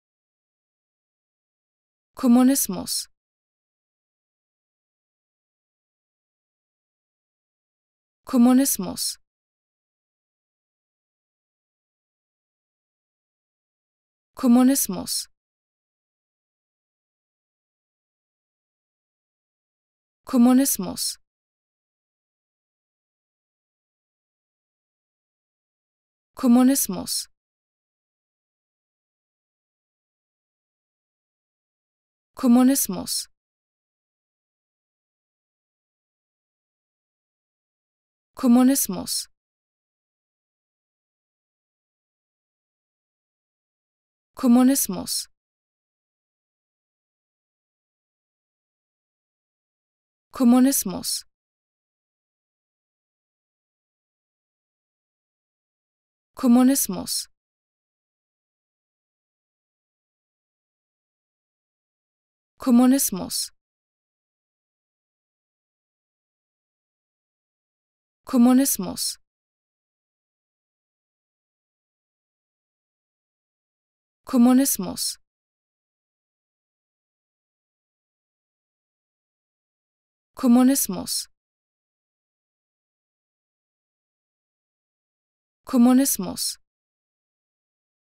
How to Pronounce ''Kommunismus'' (Communism) Correctly in German
Learn how to say ''Kommunismus'' in German with this short pronunciation tutorial.